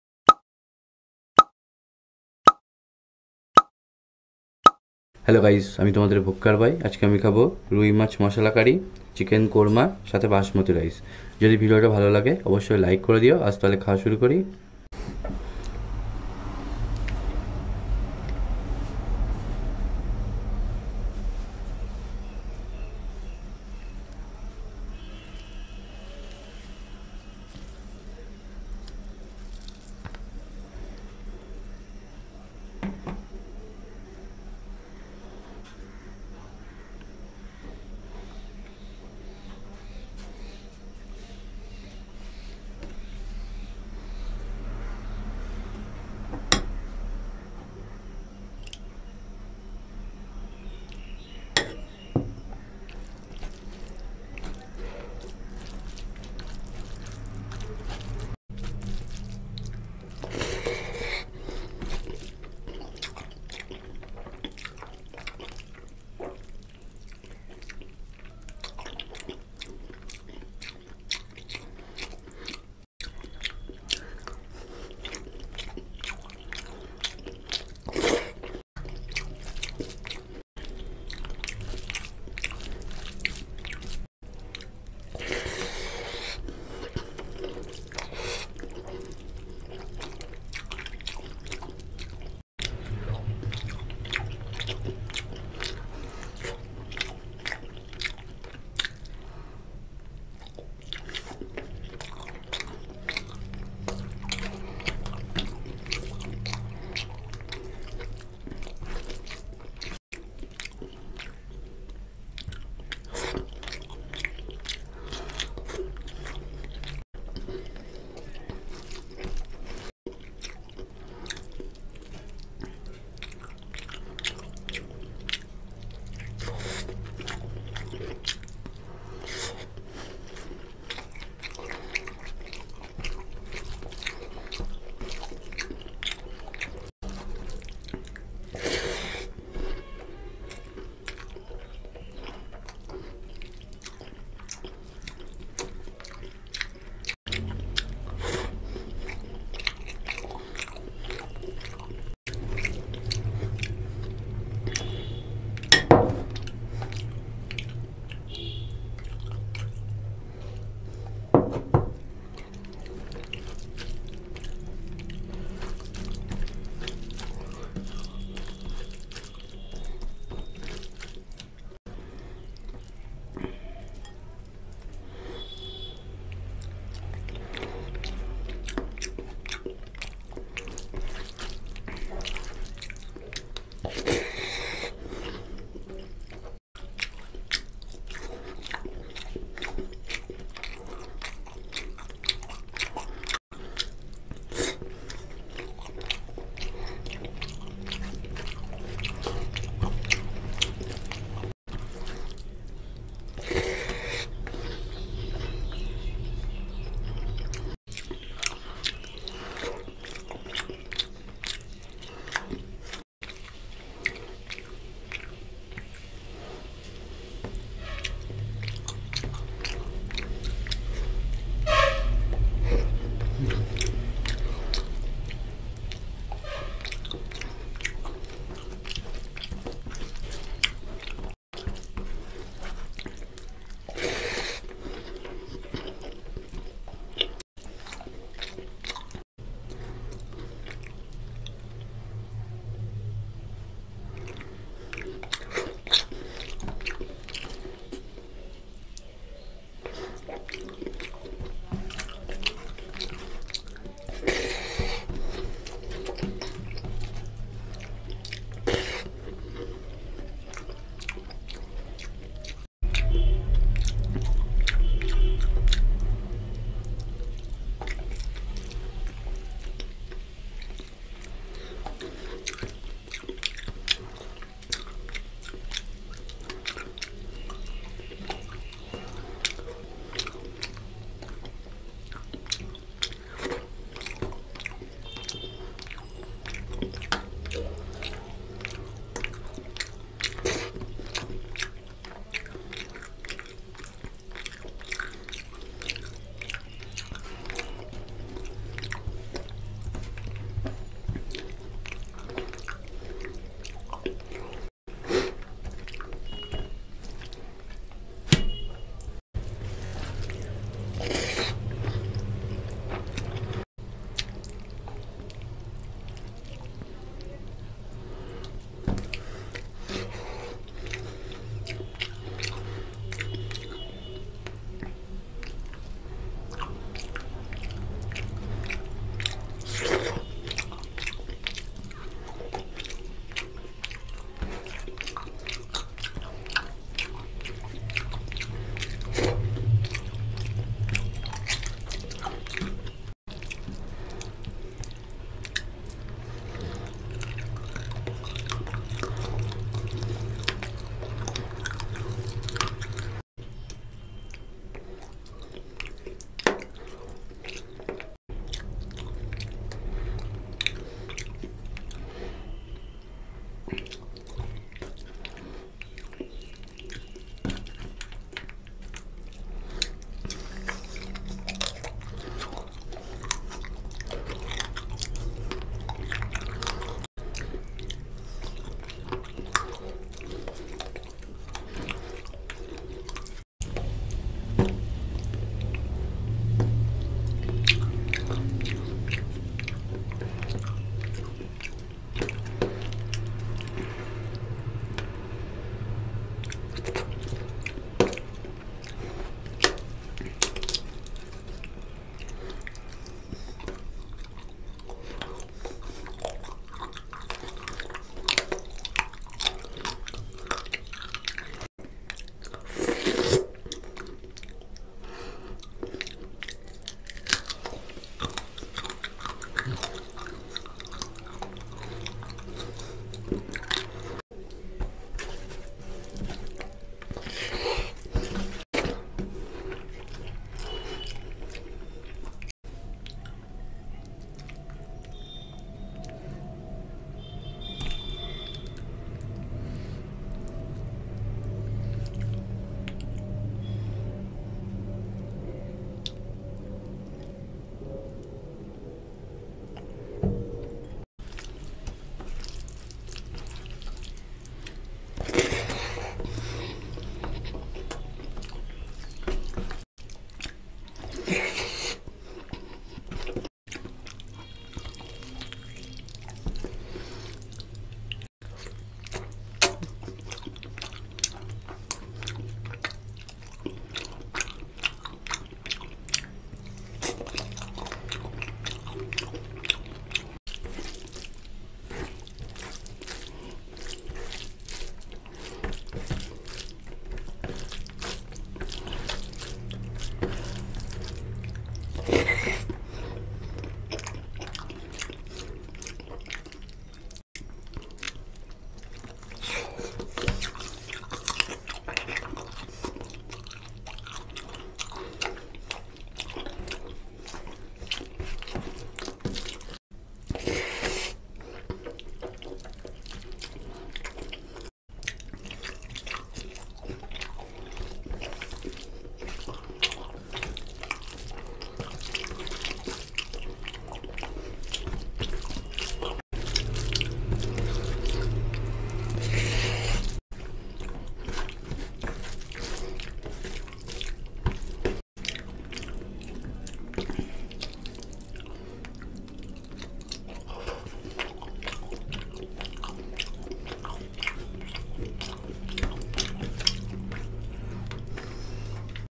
Spicy Chicken Korma Curry Eating With Basmati Rice| Full Fish Masala Curry Eating
If you like spicy food You need to taste Indian food... And , If you like eat fish You need to taste Bengali food...

Food is Heaven of Earth